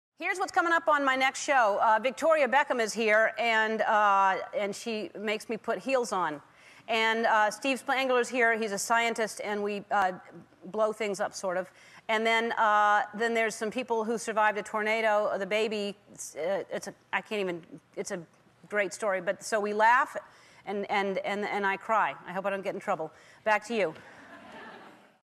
TEDS - Promo for November
Ellen talks about her first November show.